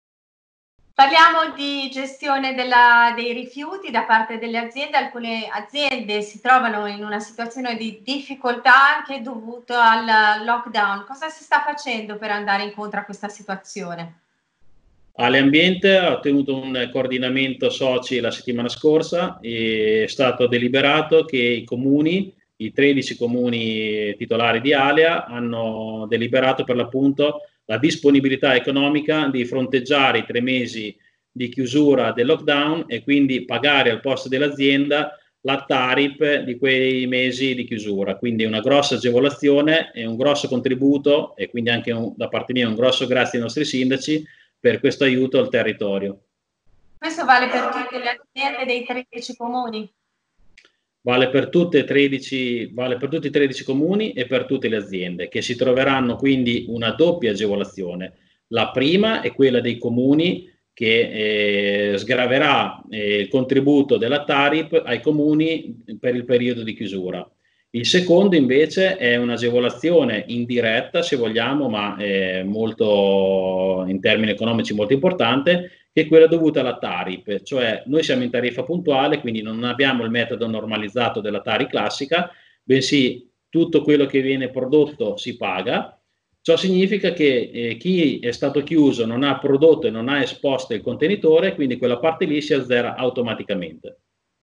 FORLI’: Alea, i Comuni si fanno carico della TARIP delle aziende per il periodo di lockdown | VIDEO
I Comuni che fanno parte di Alea Ambiente, società di gestione dei rifiuti di Forlì e comprensorio, hanno deciso che le imprese che sono state chiuse durante il lockdown non pagheranno la Tariffa Rifiuti Puntuale per l’intero periodo di inattività. Un provvedimento che coinvolge 4mila aziende del territorio.